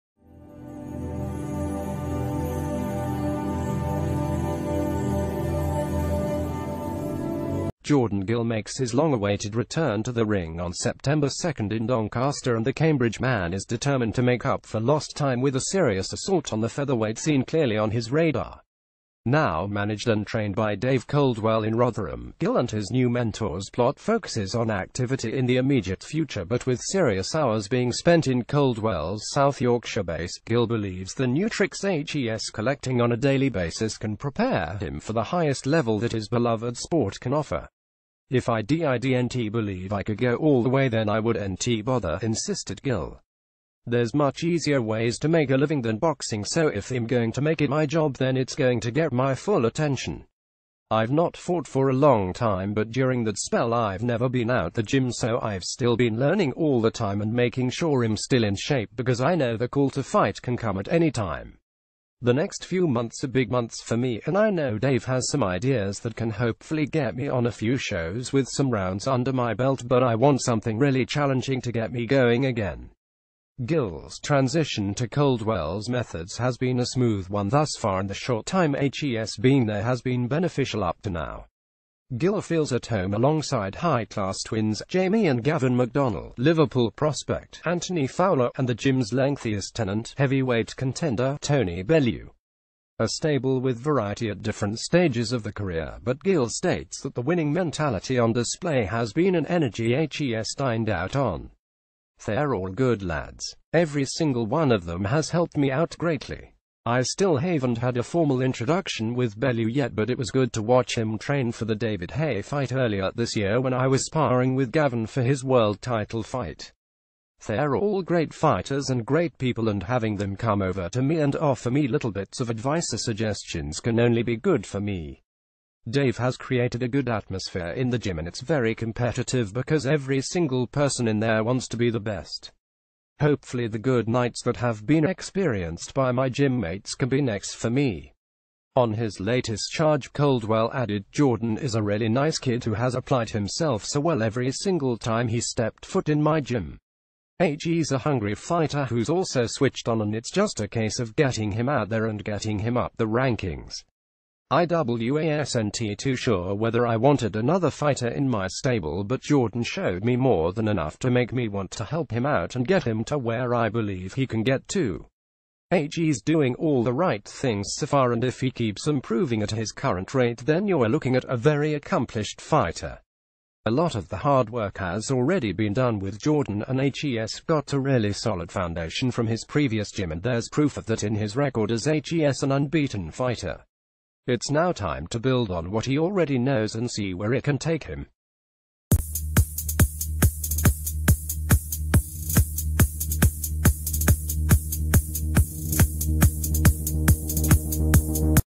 Jordan gill hooks up with david coldwell ahead of sept 2 outing
Sub